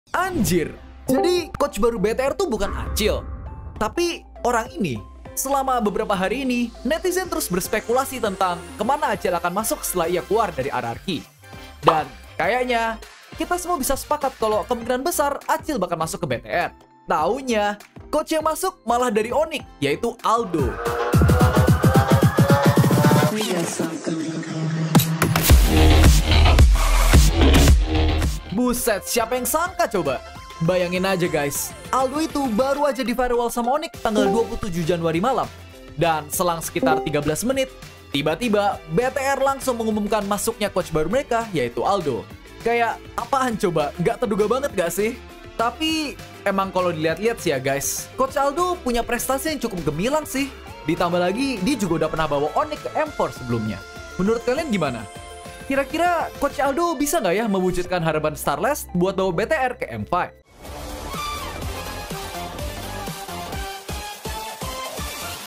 GAK MUNGKIN!!! Jadi COACH Baru BTR Bukan ACIL!?
FIX Coach BTR ternyata bukan Acil guys! coach barunya ternyata dari onic! wah bisa gak nih mimpinya starlest buat BTR menang MPl dan masuk M5 terwujud?